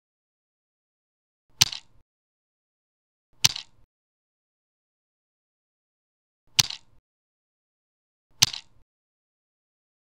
Bouton Lecture et pause | fond vert
Un simple bouton lecture et pause sur fond vert, idéal pour animer vos vidéos Youtube.

[ FONDS VERT ]

[ AUTRES RÉSEAUX ]